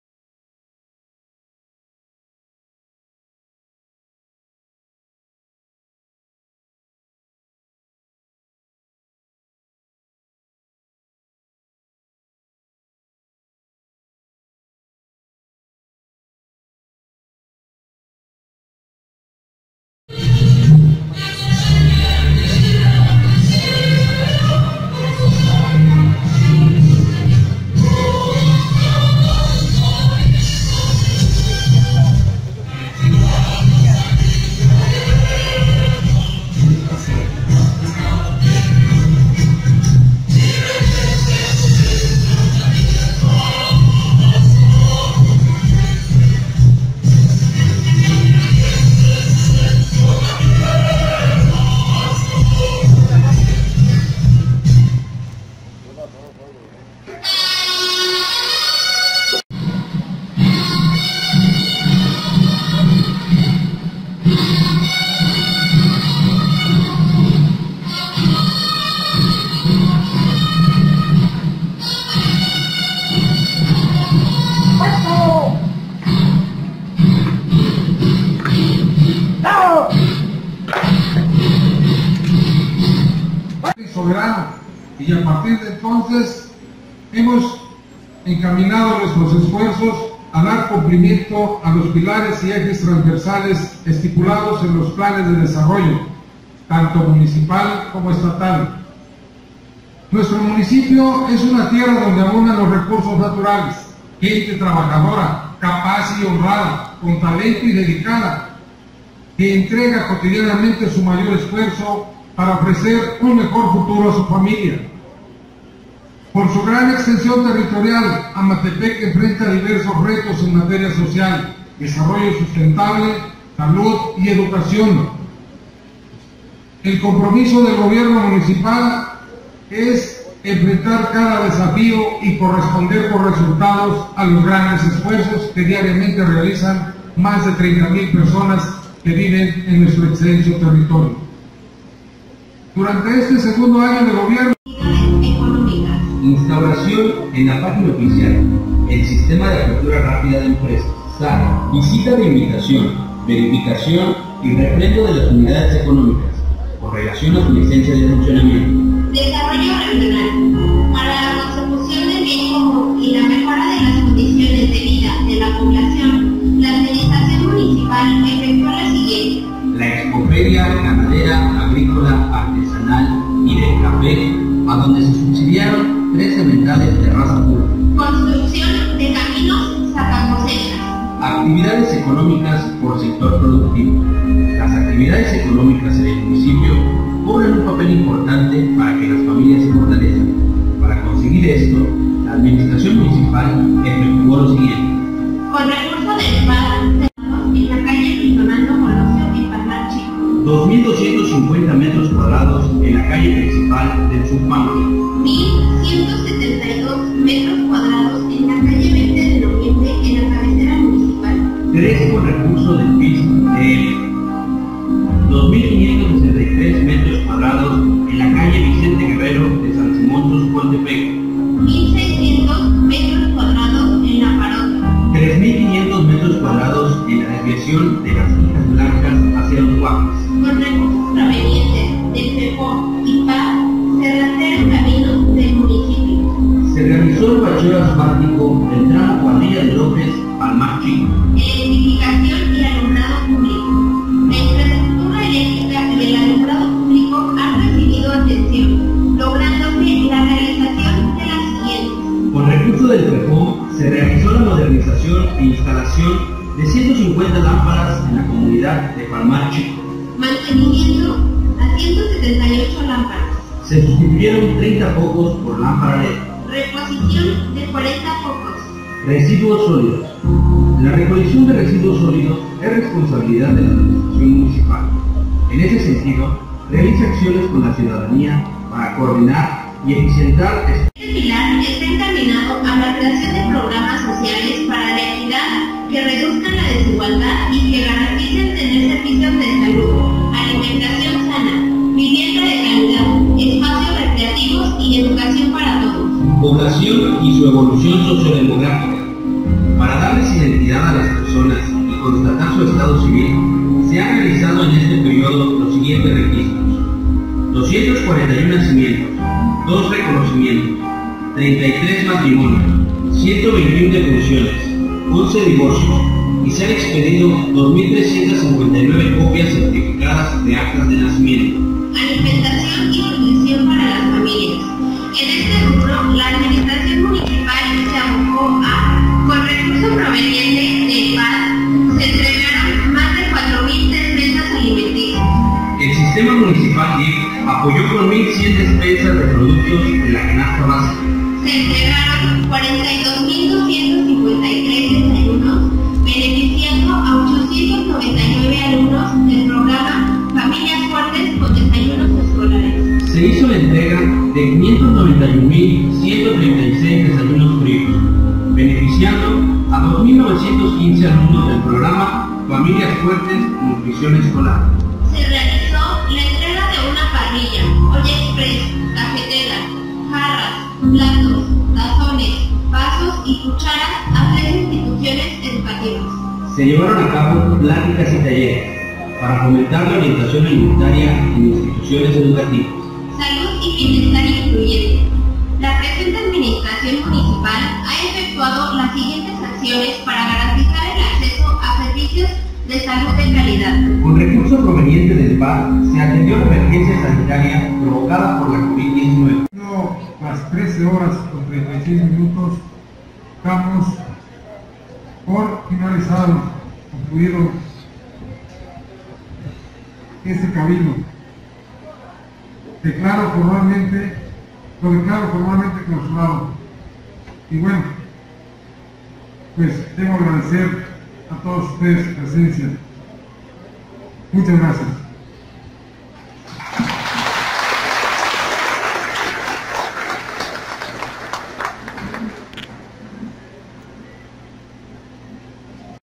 INFORME DE GOBIERNO AMATEPEC, ESTADO DE MEXICO 2020
FRAGMENTO DE EL INFORME DE GOBIERNO DE AMATEPEC ESTADO DE MEXICO AÑO 2020.